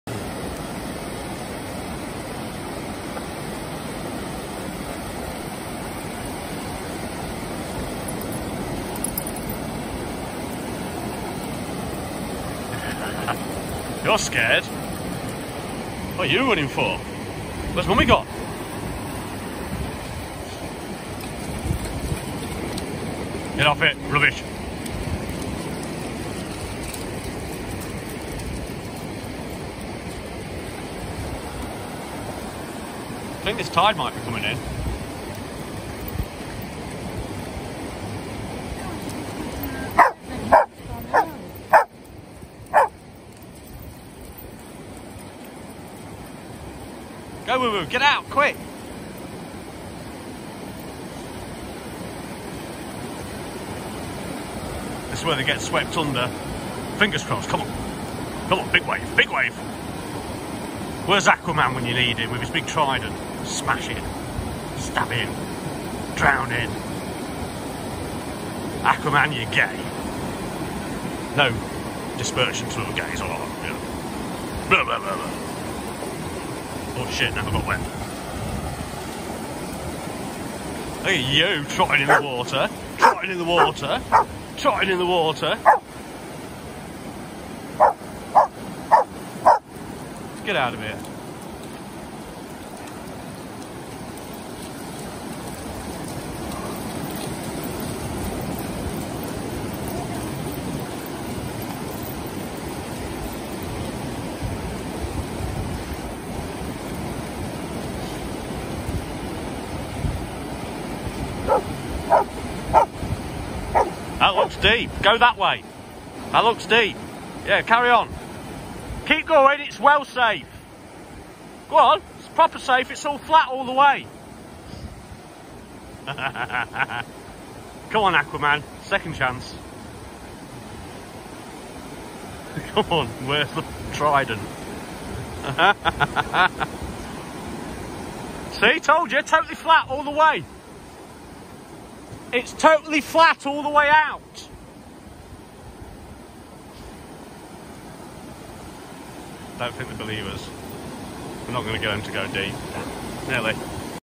Dozer The American Bulldog
At Hartlepool beach, playing in the sea